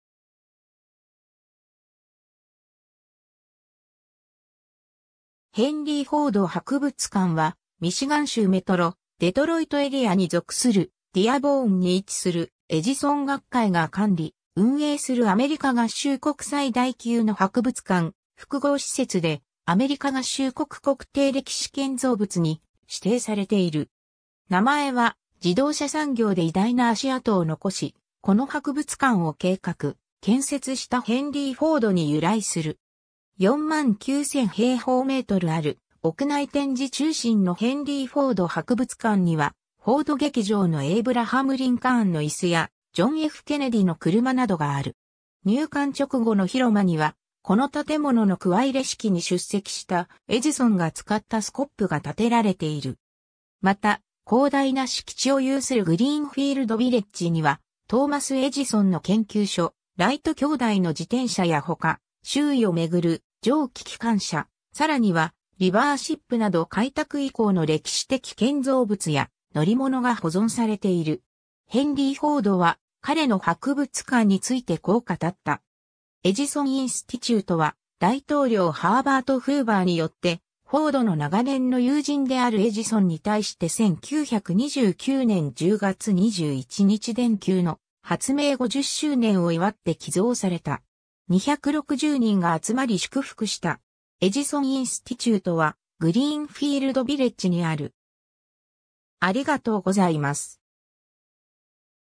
ヘンリー・フォード博物館
ヘンリー・フォード博物館（正式にはヘンリー・フォード博物館とグリーンフィールド・ヴィレッジ、英語：Henry Ford Museum and Greenfield Village）は、ミシガン州メトロ・デトロイト・エリアに属するディアボーンに位置する、エジソン学会 (Edison Institute) が管理・運営するアメリカ合衆国最大級の博物館複合施設で、アメリカ合衆国国定歴史建造物に指定されている 。
名前は自動車産業で偉大な足跡を遺し、この博物館を計画・建設したヘンリー・フォードに由来する。
 49,000 m²ある屋内展示中心のヘンリー・フォード博物館には、フォード劇場のエイブラハム・リンカーンの椅子や、ジョン・F・ケネディの車などがある。
入館直後の広間には、この建物の鍬入れ式に出席した、エジソンが使ったスコップが立てられている。
また、広大な敷地 (360,000 m²) を有するグリーンフィールド・ヴィレッジにはトーマス・エジソンの研究所、ライト兄弟の自転車屋他、周囲を巡る蒸気機関車、さらにはリバーシップ（乗船可能）など開拓以降の歴史的建造物や乗り物が保存されている。
 ヘンリー・フォードは彼の博物館についてこう語った。
 エジソン・インスティテュートは大統領ハーバート・フーヴァーによって、フォードの長年の友人であるエジソンに対して1929年10月21日電球の発明50周年を祝って寄贈された。
260人が集まり祝福した。
 エジソン・インスティテュートはグリーンフィールド・ヴィレッジにある。